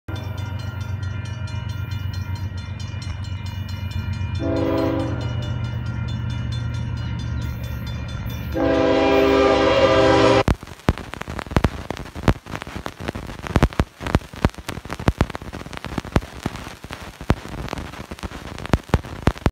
Up 7793 power move going through Pleasanton
Up 7793 leads a power move through pleasanton